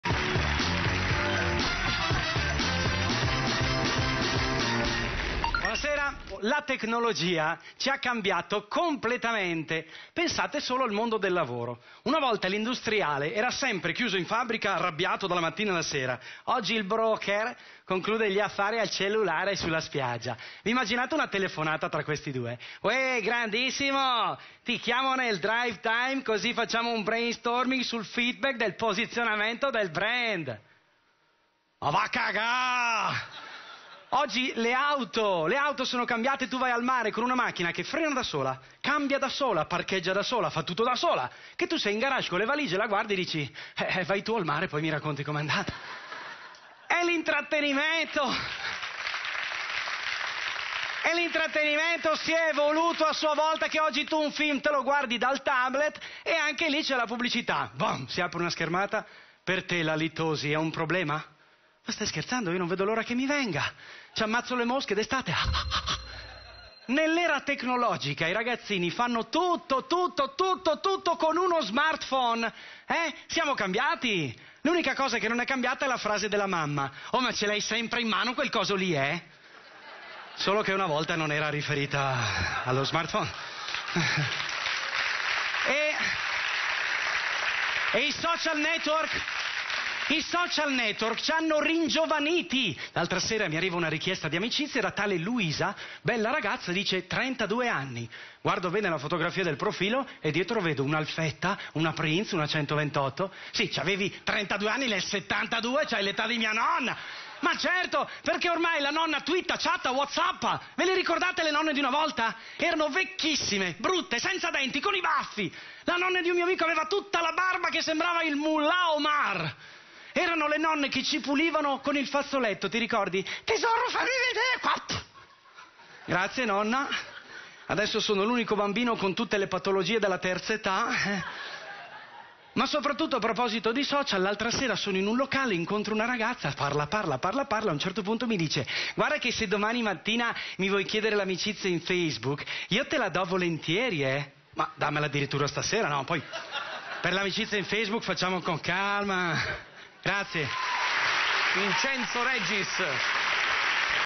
Vinceno Regis, monologhista
Vincenzo Regis ci parla del vasto e oscuro mondo della tecnologia...